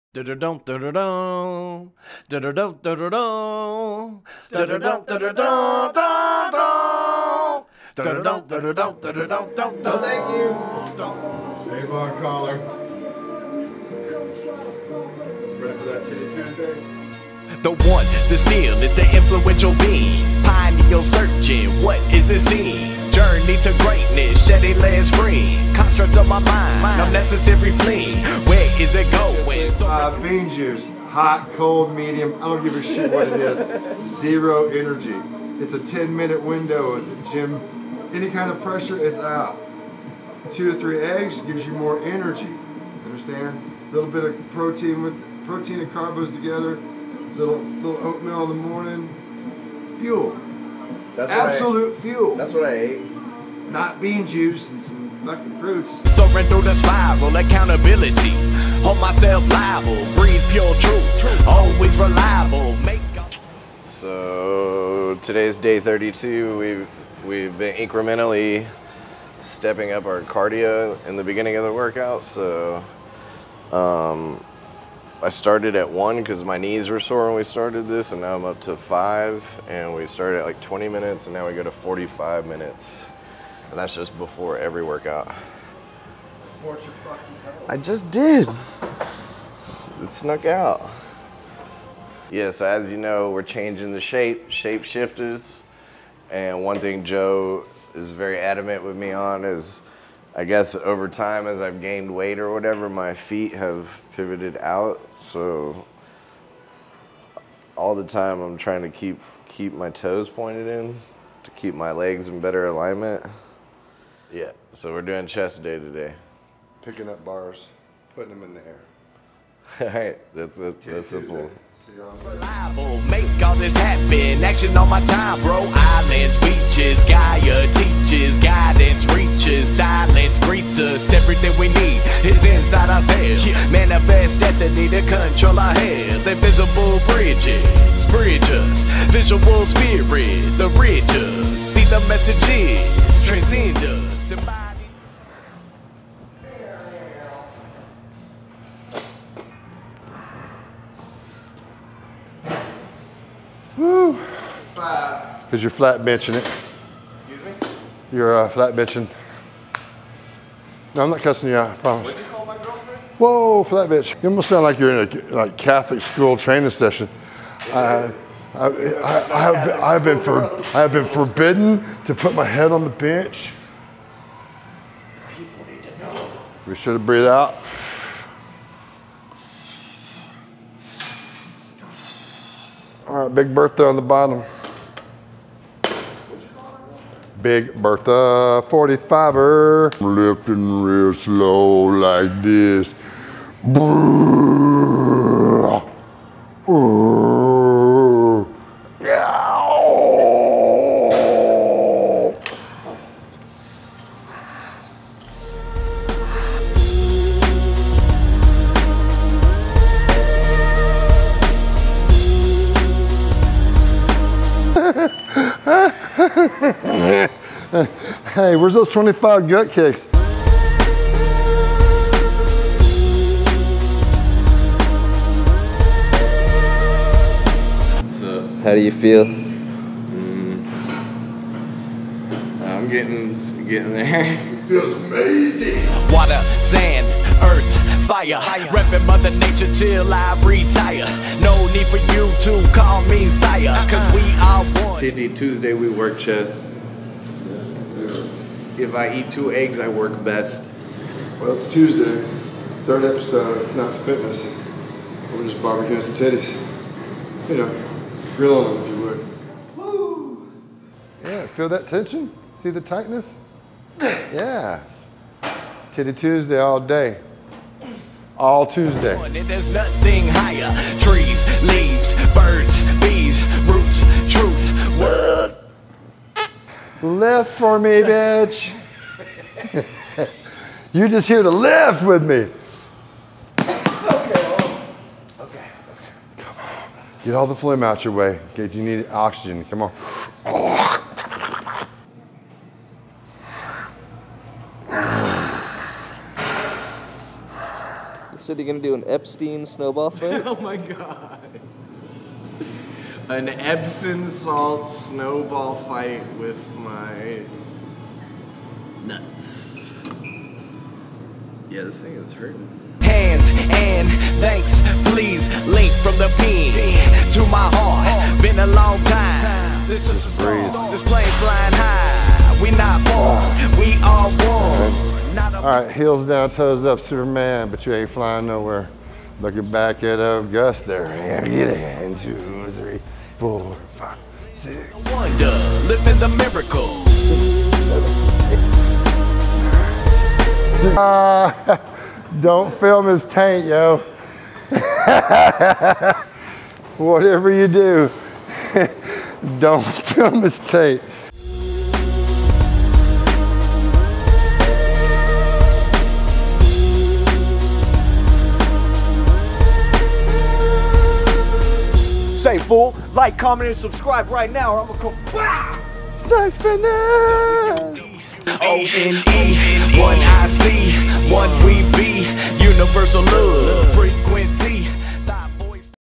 SnipesFitness 100 Day Challenge - EP 4 - Day 24 Titty Tuesday
EP 4 - Day 24 - Joe & I get busy on them titties! I have made a commitment to workout everyday and not drink alcohol for 100 Days! With the help of my friend "Bosu Joe", we are doing the SnipesFitness 100 Day Challenge to see how much we can transform our bodies (and mind+soul) in 100 days time. I am inviting you to join along with us, 100 Days - No Booze and Move that Body Everyday! Do It - Time Is Now - Let's Go!

JOIN ME FOR THE SNIPESFITNESS 100 DAY CHALLENGE!!! 😅💪🌀🌱

for all things Snipes!

We Linked Up On Social Media???